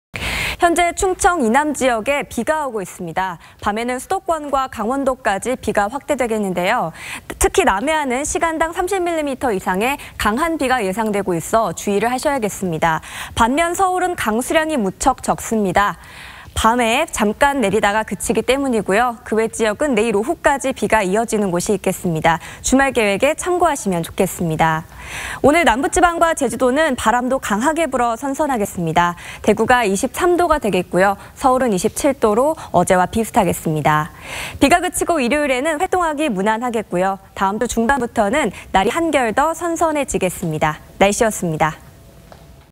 [날씨] 오늘 전국 대부분 가을비…남부·제주 강풍 주의
현재 충청 이남 지역에 비가 오고 있습니다. 밤에는 수도권과 강원도까지 비가 확대되겠는데요, 특히 남해안은 시간당 30mm 이상의 강한 비가 예상되고 있어 주의하셔야겠습니다.